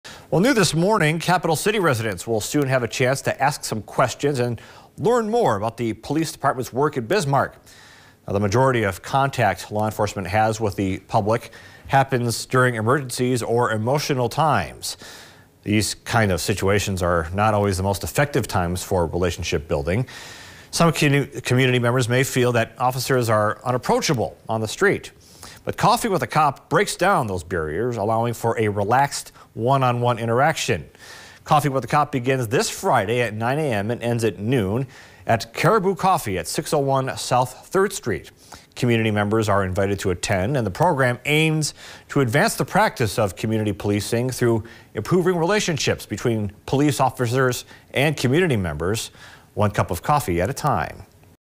Coffee with a cop is back
Coffee with a Cop breaks down barriers with their community and allows for a relaxed, one-on-one interaction over a cup of coffee.